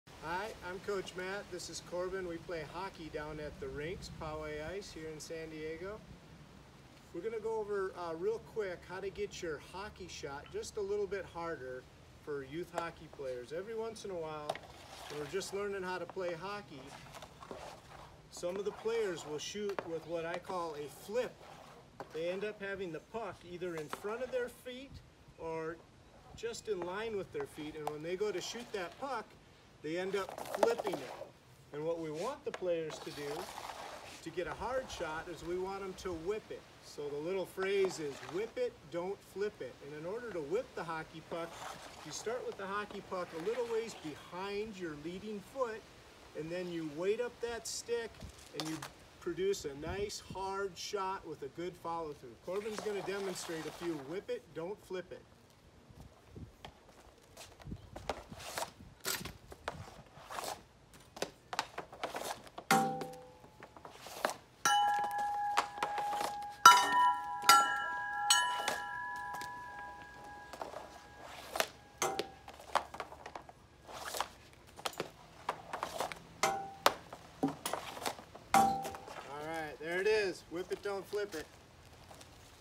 Hockey At Home Off-Ice Tips: Whip it, Don't Flip it!
Coach Matt and Corbin from our Poway ICE location with a tip for youth players to get a stronger shot!